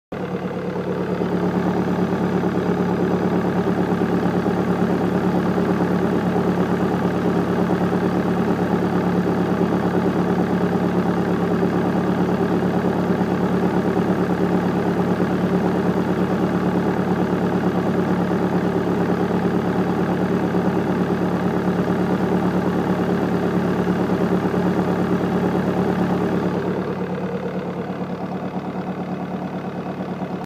AAZ swapped caddy, Pulley problem?
Just finished my AAZ swap to my Caddy, would this be considered the start to a wobbly crank pulley problem? Engine has low life of roughly 130km, crank bolt remains at torque spec. only removed the serpentine belt pulley prior to this observation.